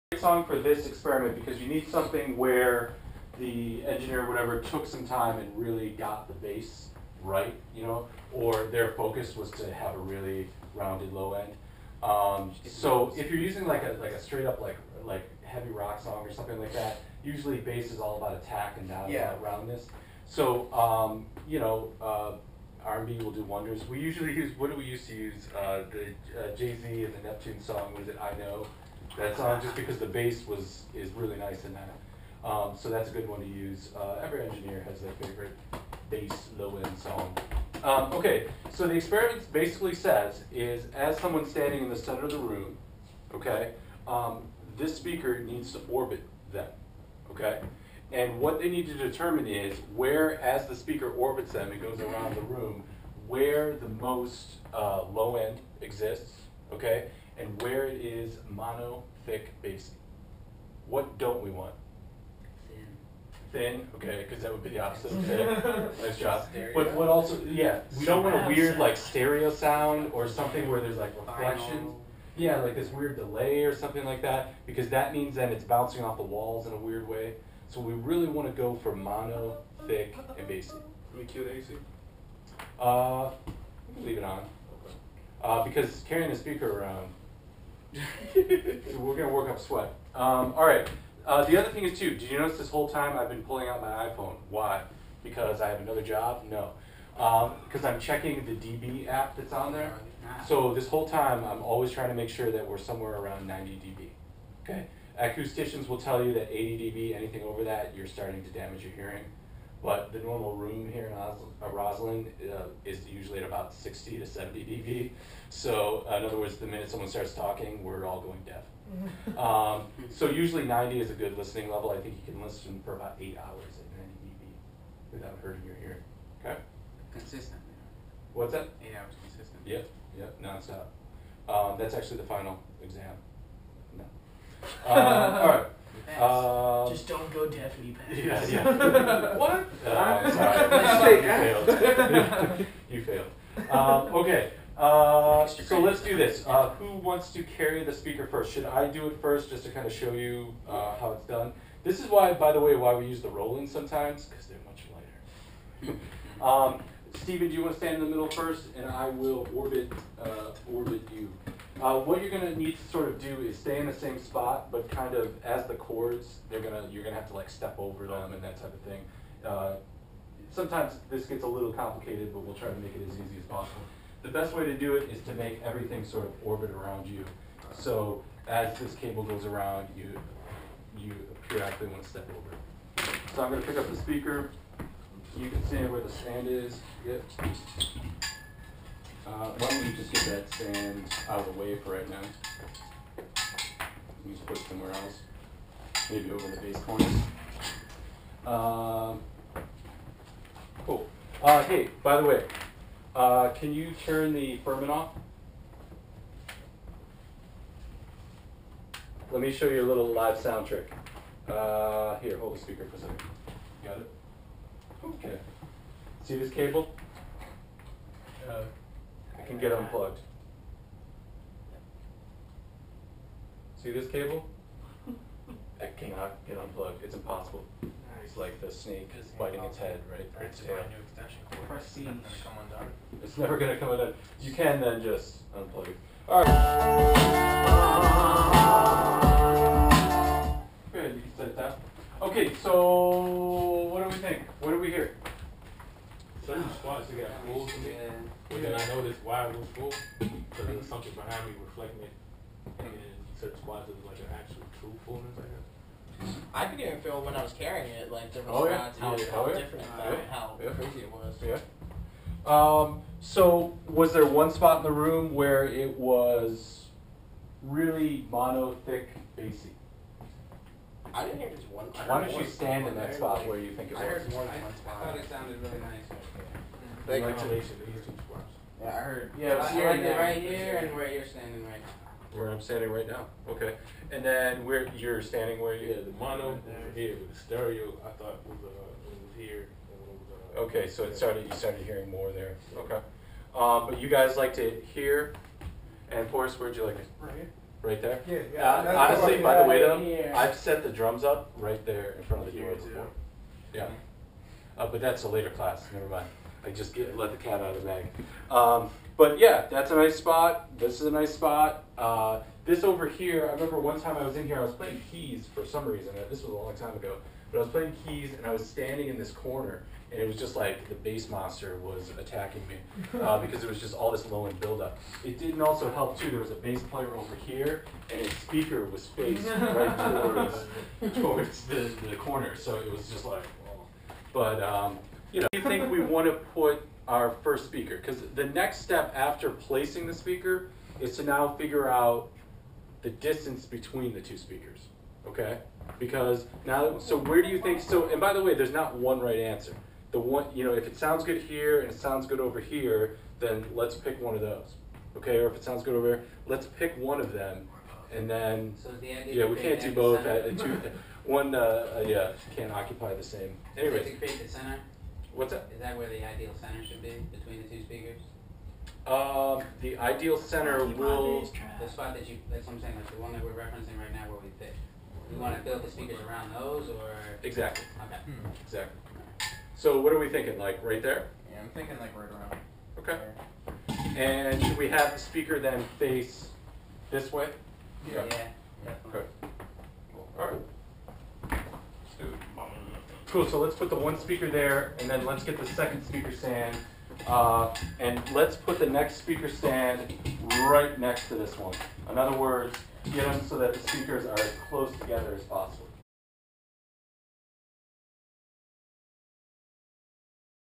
Stavrou's Speaker Placement Method - Professor dB
Advance recording experiment to teach students Micheal P. Stavrou's speaker placement method for profession and home studios. Filmed in Studio A at AIW.